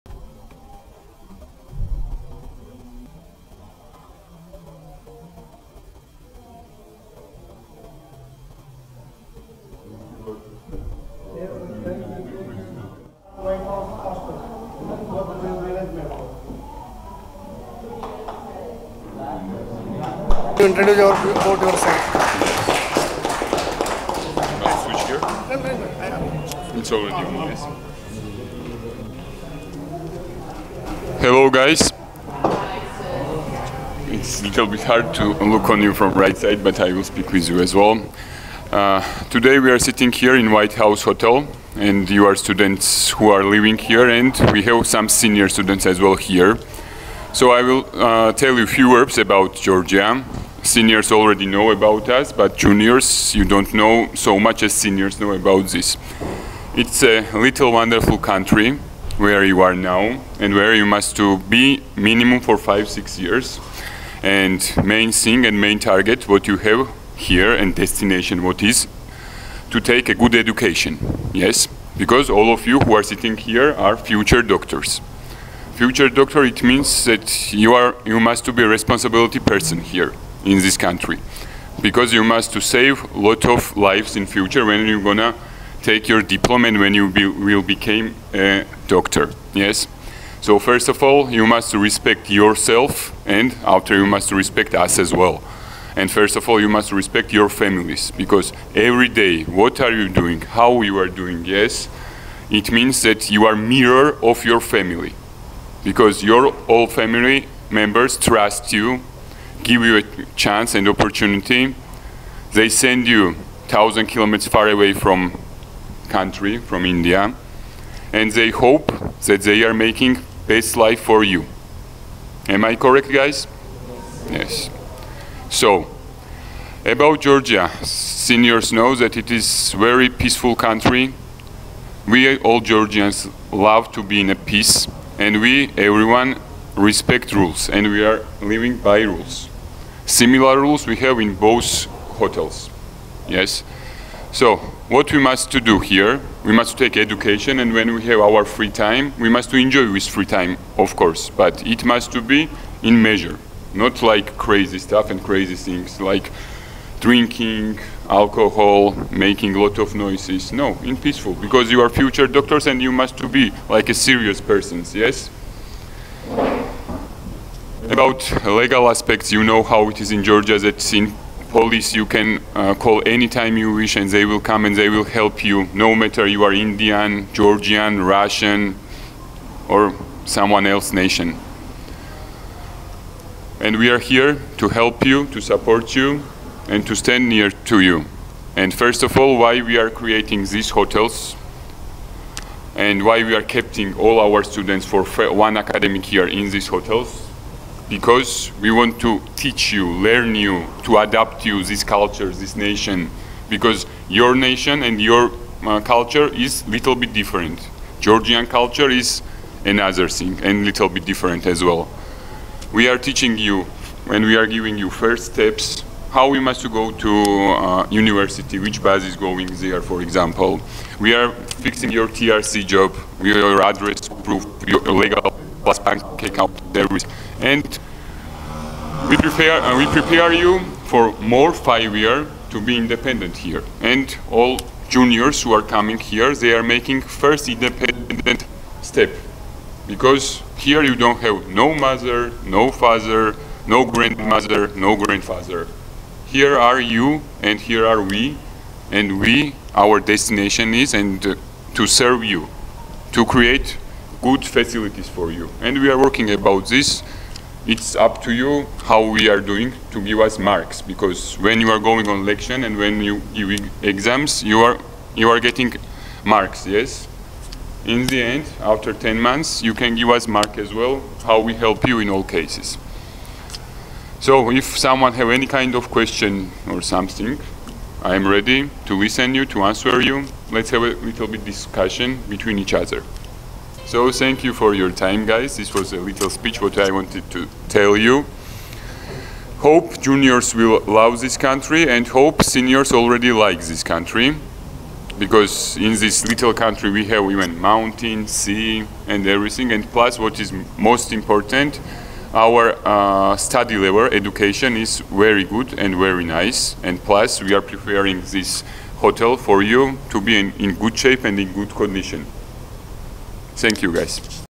Mr.Temo with Karnataka Medical Students | Georgia Rules & Regulations | White House Hostel @ Tbilisi
Mr.Temo with Karnataka Medical Students from Medicon Overseas. Georgian American University South Indian Students White House Hostel @ Tbilisi, Georgai.

Georgia rules and regulations form MBBS students.